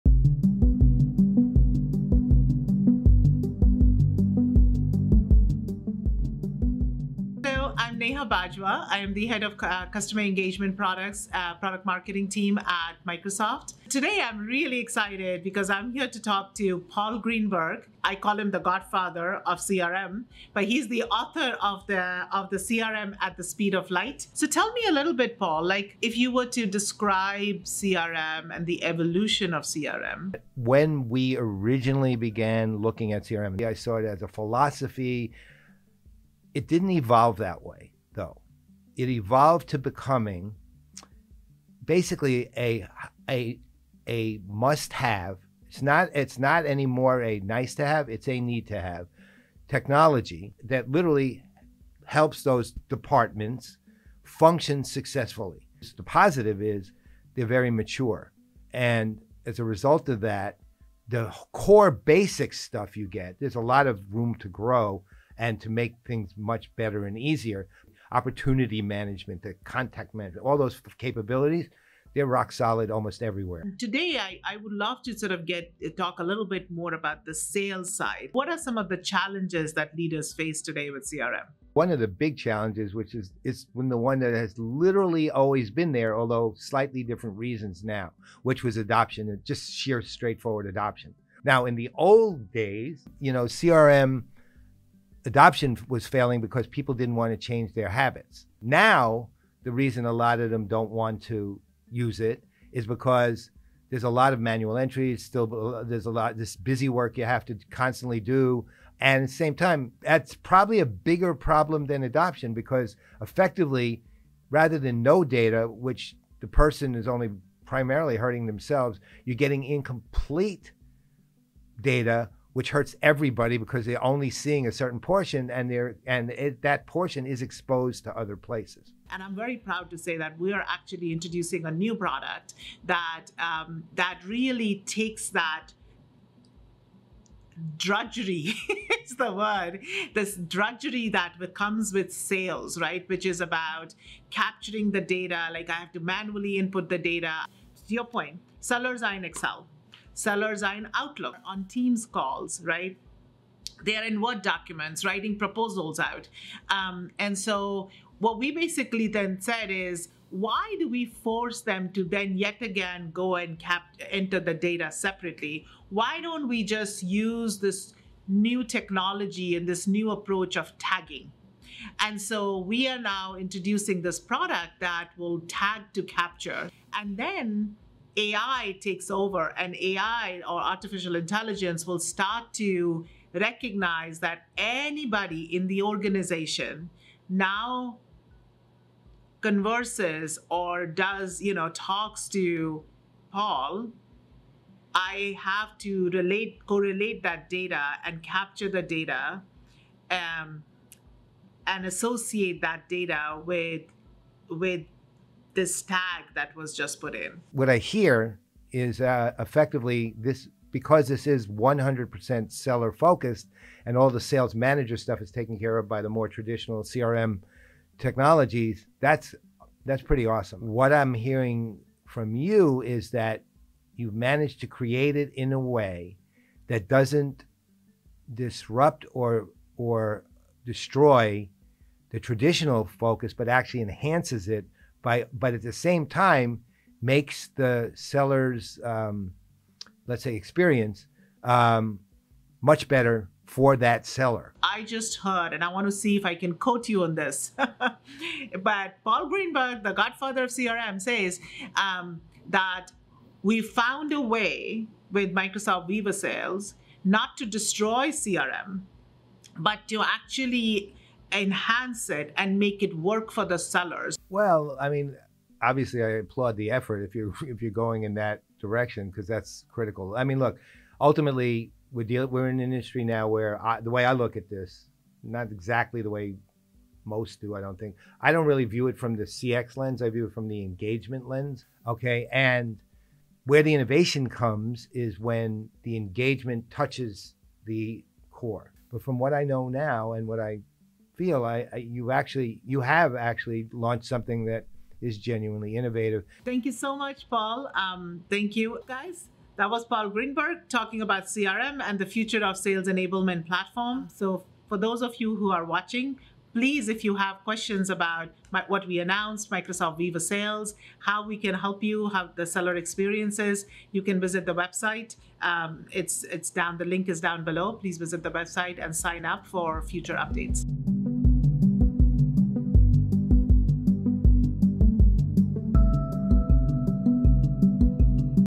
The future of sales enablement with Paul Greenberg
Hear from Paul Greenberg, the author of the best-selling CRM at the Speed of Light, on his thoughts and insights on the future of sales enablement.